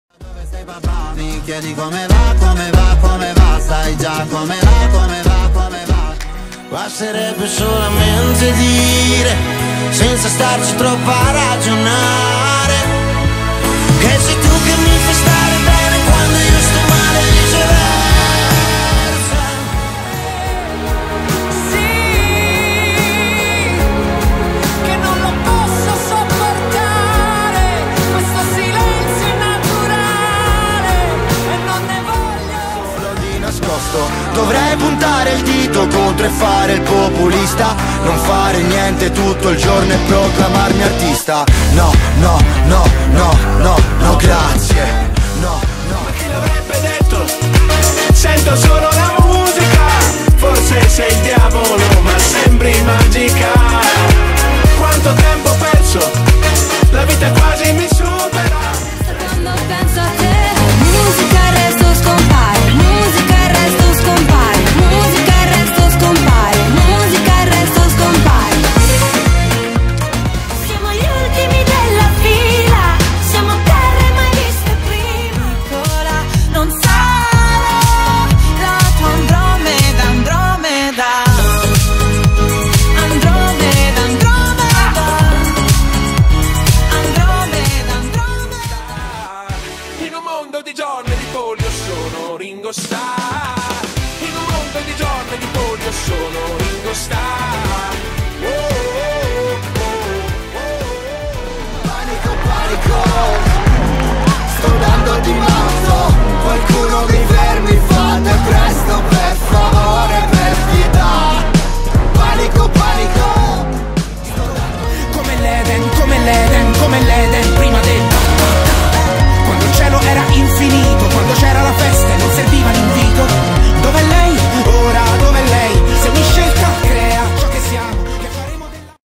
EUROVISION 2020 - ITALY - SANREMO - MY TOP 10
Hi! This is my top 10 of Sanremo!